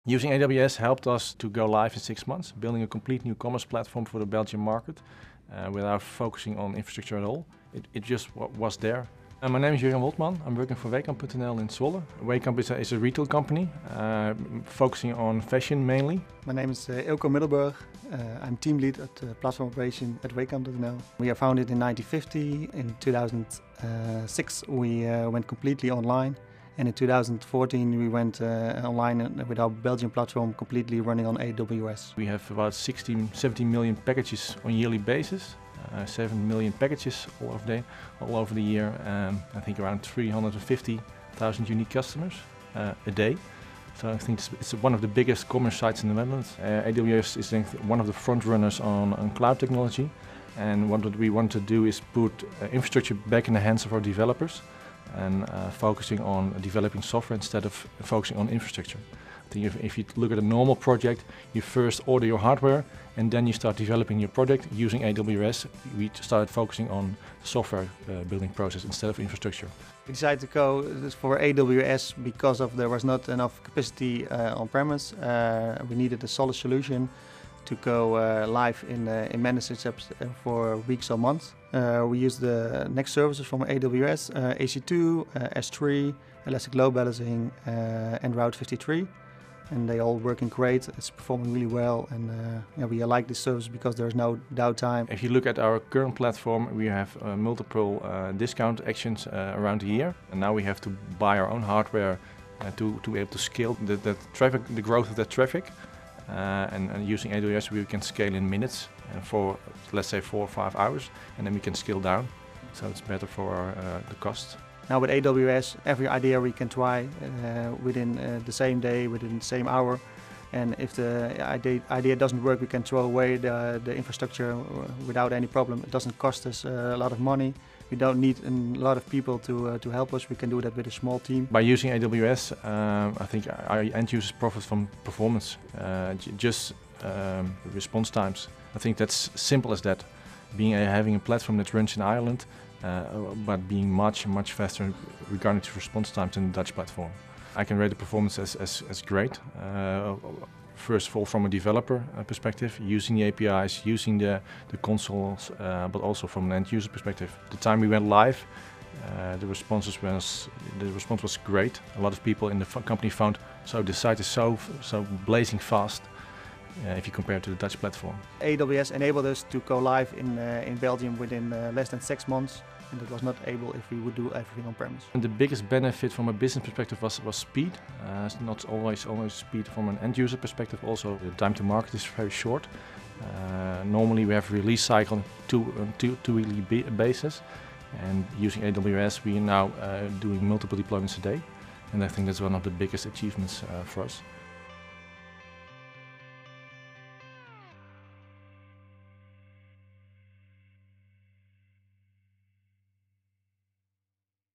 Wehkamp Uses AWS to Rapidly Deploy, Update E-Commerce Platform in New Market
Wehkamp is the largest e-commerce retailer in the Netherlands. In a move to expand into Belgium, it wanted a new cloud platform that was more agile and responsive than the IT system used to run its original site. It turned to AWS, using a number of services for its testing, development, and production environments.

By using AWS, Wehkamp was able to deploy its new platform in about six months—compared to 12-18 months using its prior IT platform—and can delivery updates and new features several times a day, instead of one every two weeks as it did in the past. It also now has a highly scalable platform that easily manages seasonal spikes in traffic.